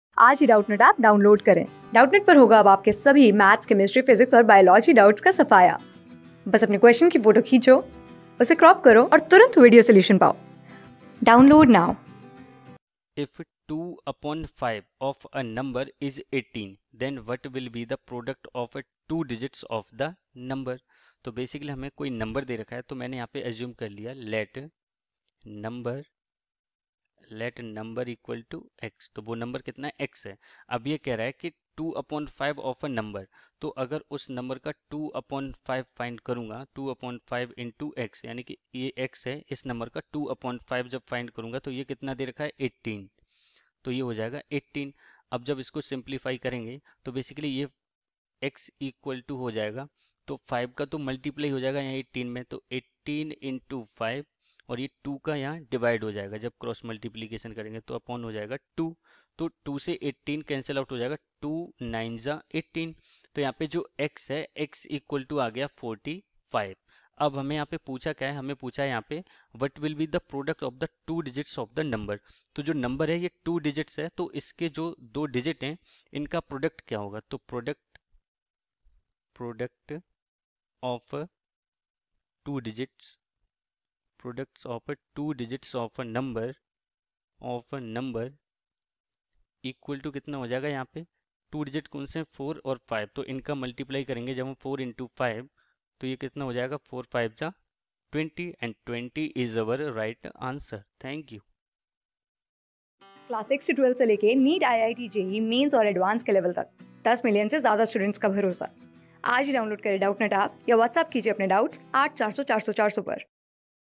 If 2/5 of a number is 18. What will be the product of two digits of the number? | CLASS 6 | ALG...
If 2/5 of a number is 18. What will be the product of two digits of the number?
Class: 6
Subject: MATHS
Chapter: ALGEBRA
Board:FOUNDATION

👉 Have Any Query? Ask Us.

Our Telegram Pages:
class 6
6 class ka math syllabus